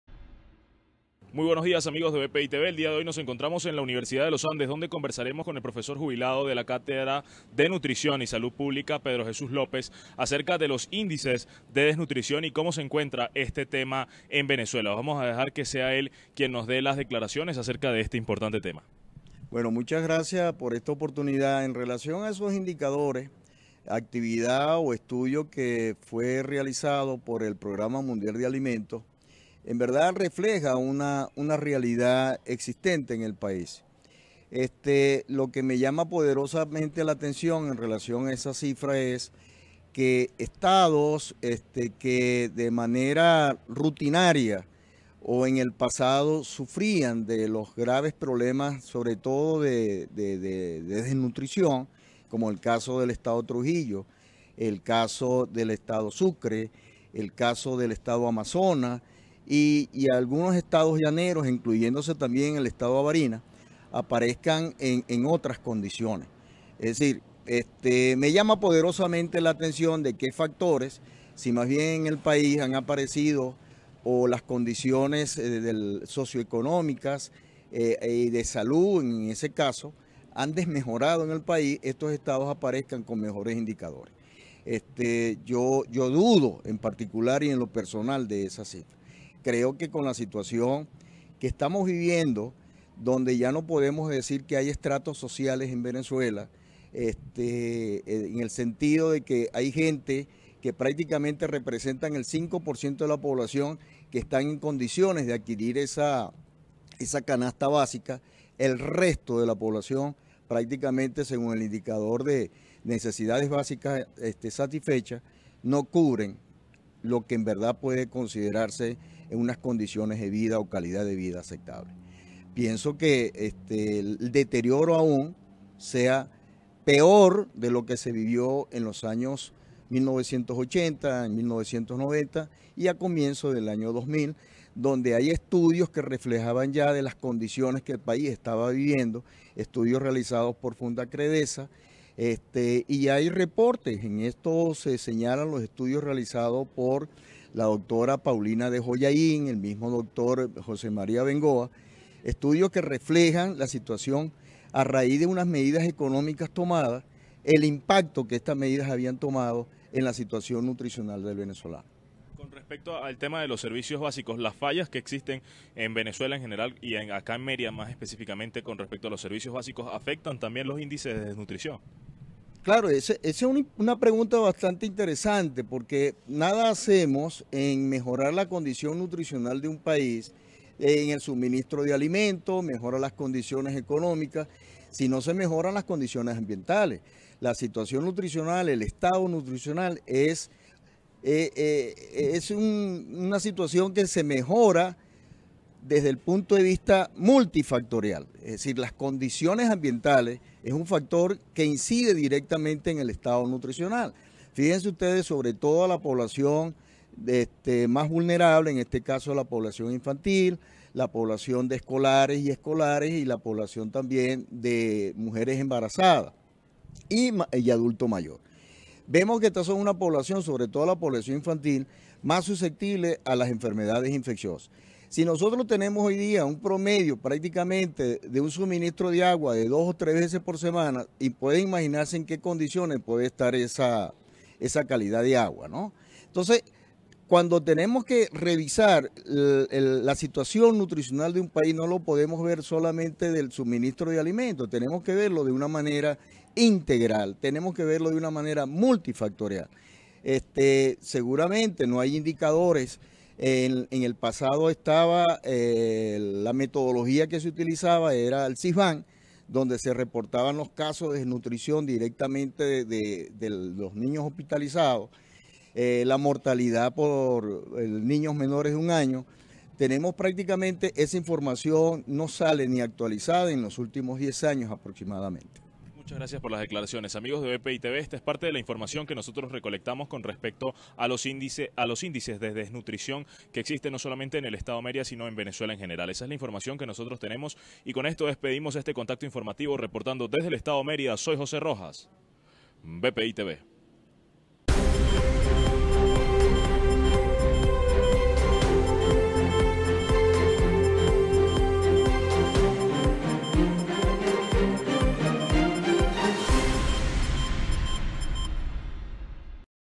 Mérida - Productos de las canasta básica es difícil de adquirir - VPItv
Pedro Jesús López, profesor de nutrición de la Universidad de los Andes, indicó que debido a la crisis que sufre el país la mayoría de los venezolanos se les complica adquirir los productos de la canasta básica.